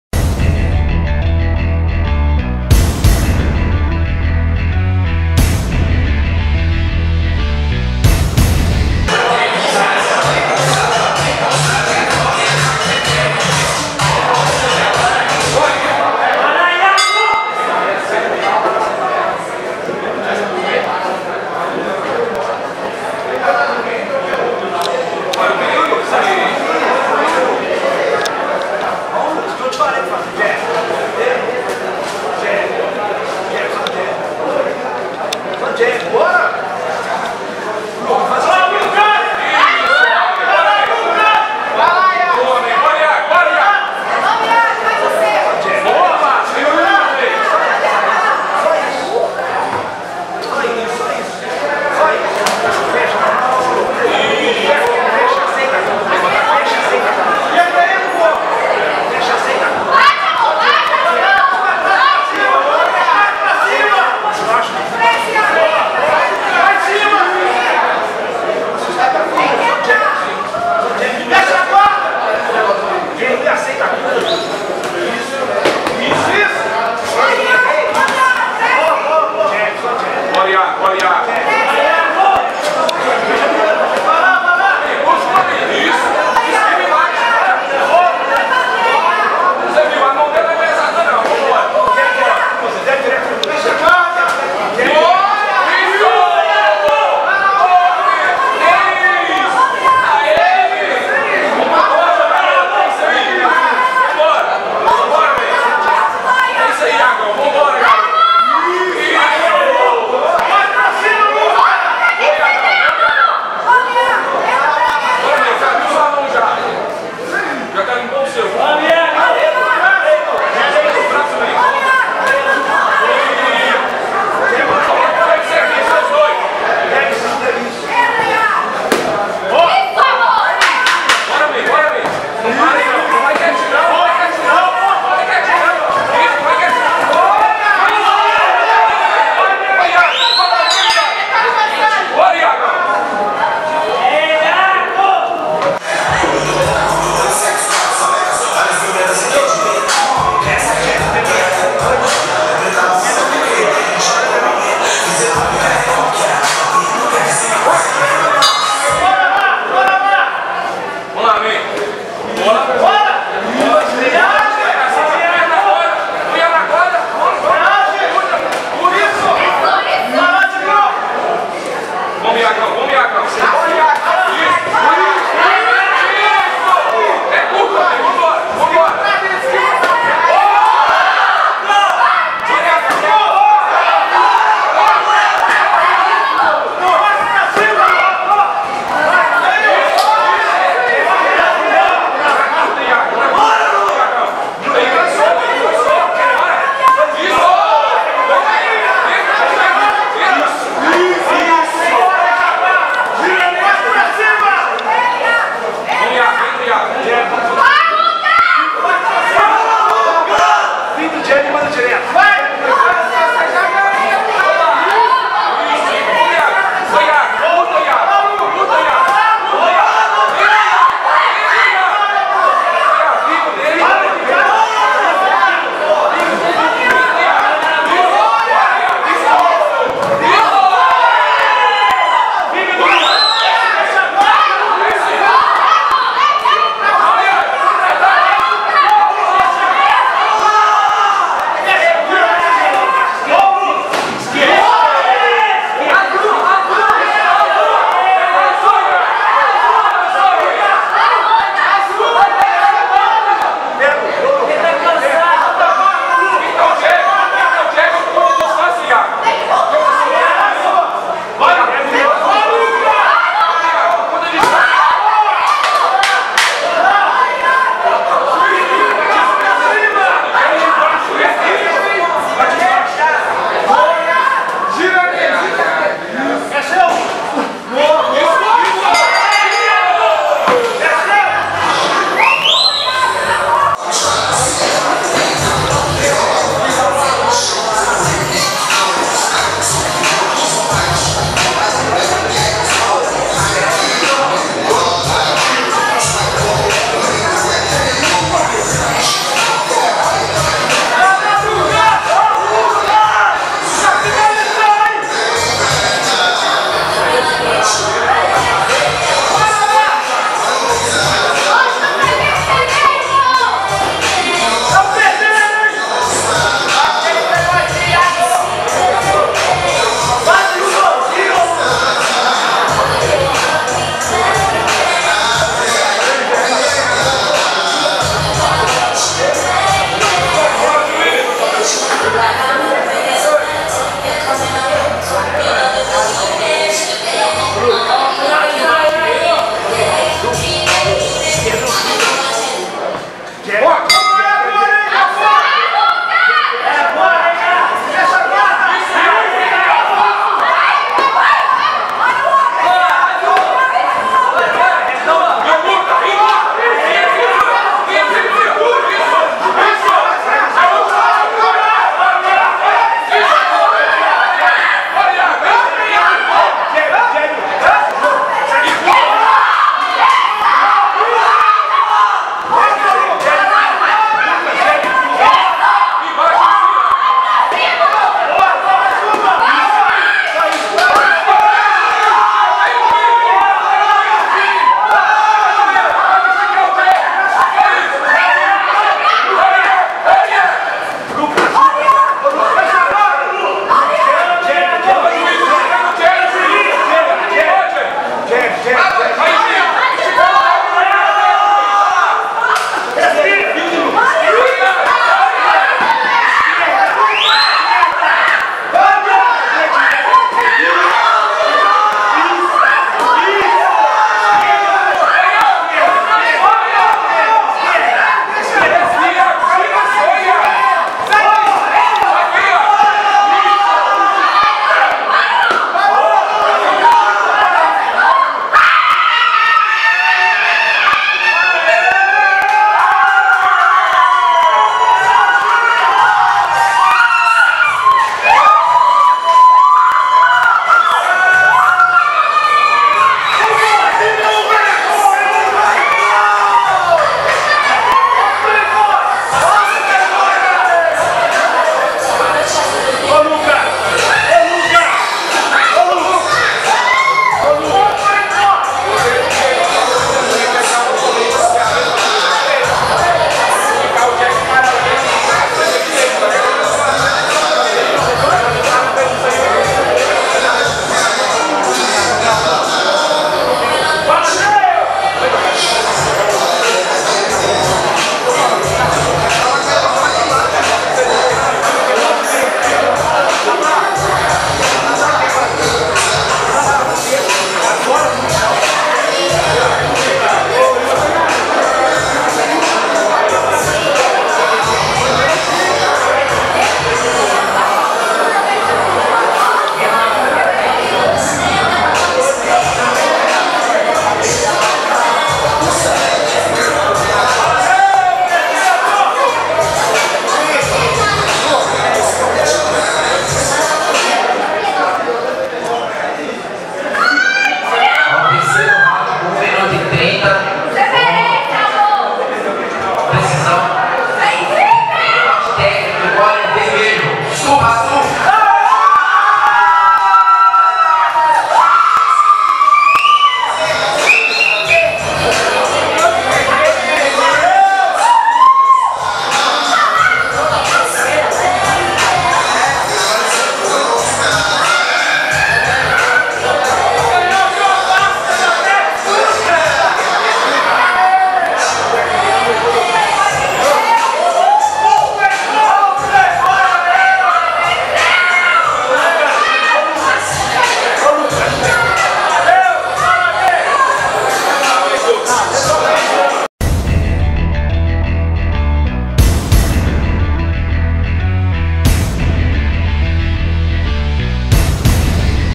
Lucas Von vs Hiago - Boxe Evolution 3
Lucas Von (JC Simão) venceu Hiago Aparecido (LG Fight) por nocaute técnico no terceiro round - 81kg.